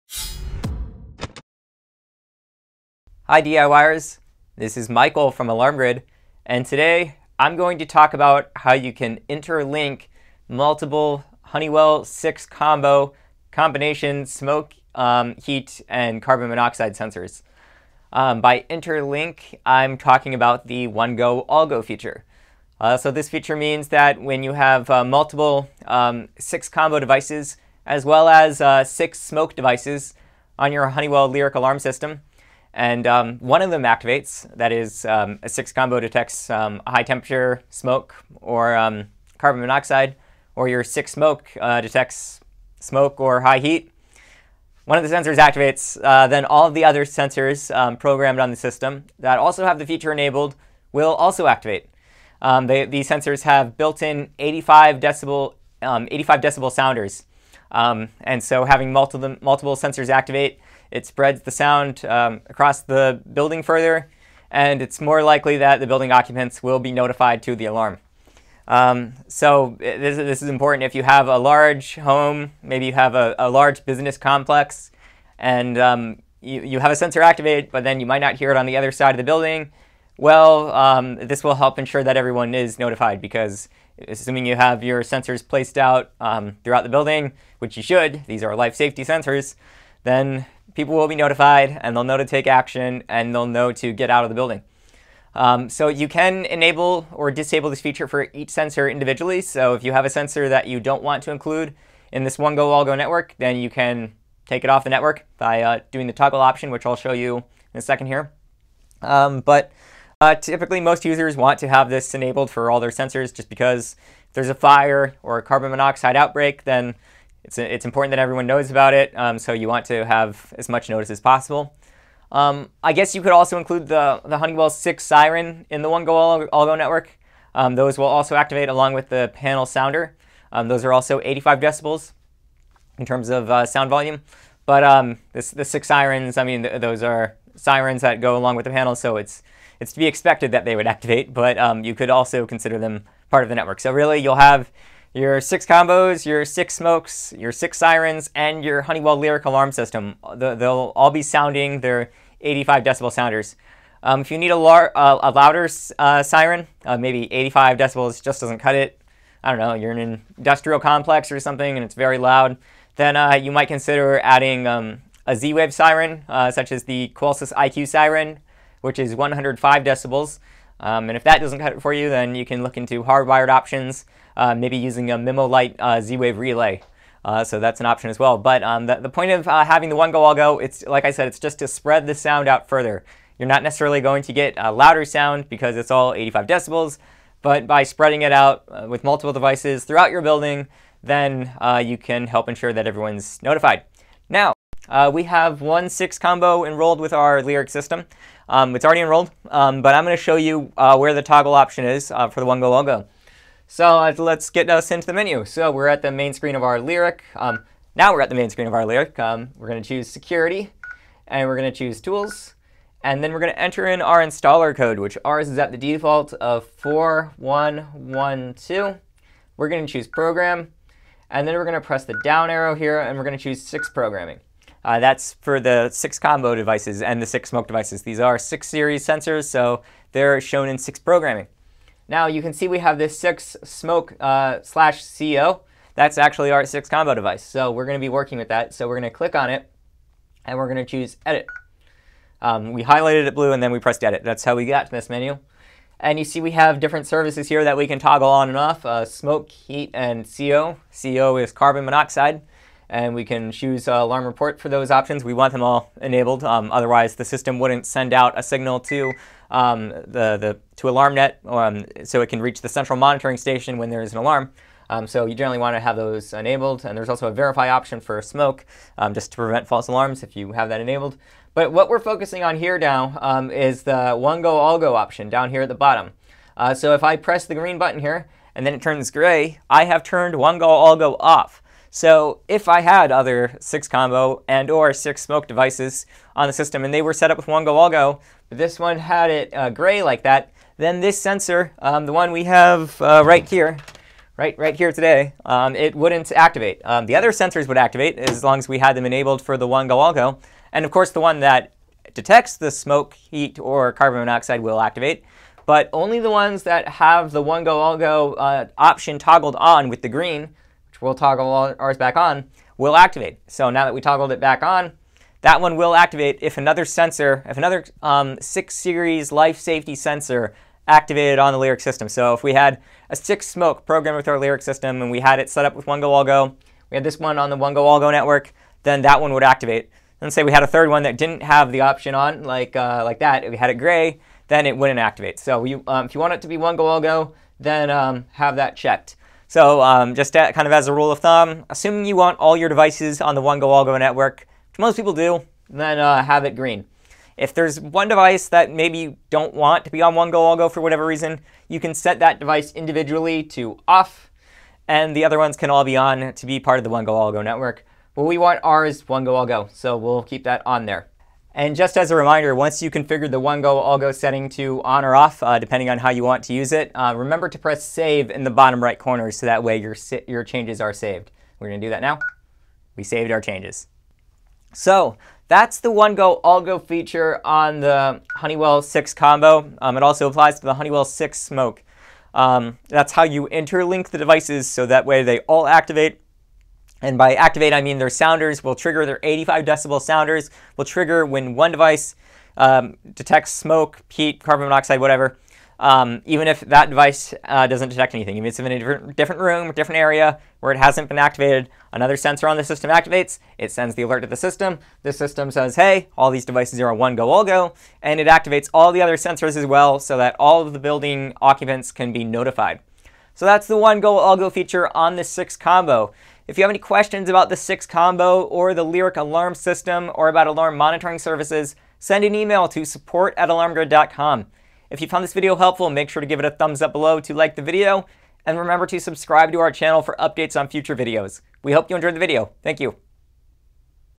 Interlinking Honeywell SiXCOMBO Devices
In this video, Michael from Alarm Grid shows how you can interlink multiple Honeywell SiXCOMBO devices on the same Honeywell Lyric System so that they all begin sounding when only one of them is activated. This feature is commonly referred to as One-Go-All-Go. This is an important safety feature that helps ensure that all building occupants are alerted during a potentially life-threatening emergency. You can include all programmed SiXCOMBO devices and SiXSMOKE devices in the One-Go-All-Go network. You have the option of excluding some devices from the One-Go-All-Go network while including others if you want. However, there's very little reason not to have all your SiXCOMBO and SiXSMOKE sensors operating as One-Go-All-Go.

The Honeywell SiXCOMBO and Honeywell SiXSMOKE devices have 85 dB sounders built inside. These 85 dB sounders are actually just as loud as the 85 dB sounder inside the Honeywell Lyric Alarm Panel. But the advantage to setting up a One-Go-All-Go network with all your SiXCOMBO and SiXSMOKE devices is that it will spread the sound out across a larger area. It may not make the sound any louder, but you can have a sounder in multiple rooms and hallways spread throughout the building. This can be very important if the system is being used in a larger home or business. Not to mention, any Honeywell SiXSIREN devices that you have set up with the system will also begin sounding. The SiXSIREN devices also have 85 dB sounders built inside.

For all programmed SiXCOMBO and SiXSMOKE devices, there is a toggle option within Honeywell SiX Sensor programming for One-Go-All-Go. You can turn it ON or OFF for each individual sensor that is enrolled with the Lyric Alarm System. In most cases, a user will simply enable the One-Go-All-Go feature for each SiXCOMBO and SiXSMOKE device that is enrolled with the Lyric, as there is virtually no reason to want to have every sensor begin sounding in the event of a fire or an outbreak of carbon monoxide gas.